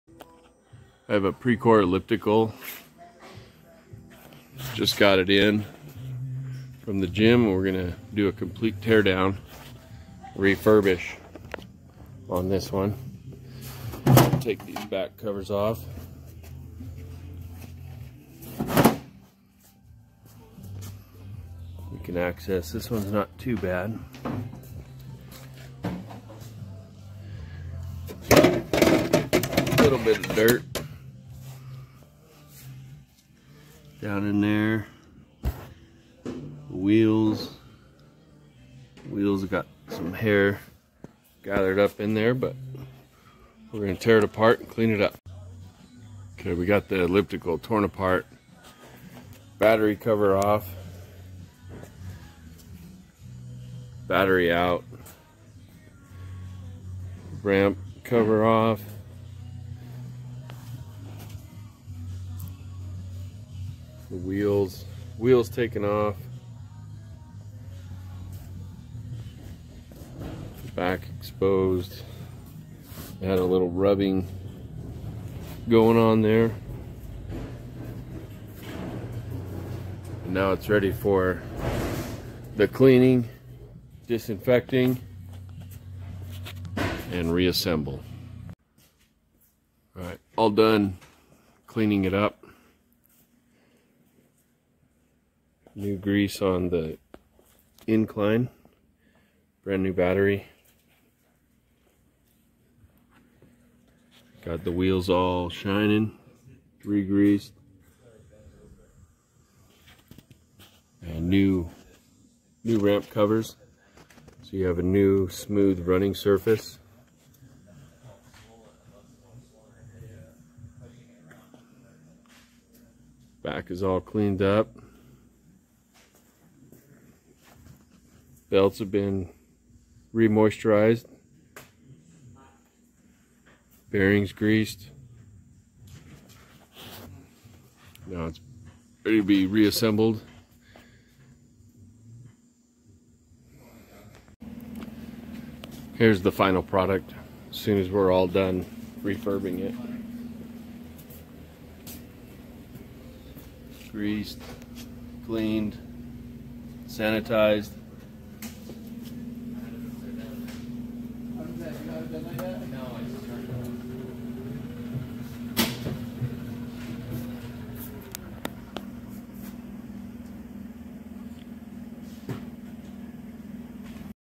Precor Elliptical Refurbishment Process
We are showing you how to break it down and put it back together, this one didn't need anything major replaced, that’s the difference between refurbishment and reconditioning.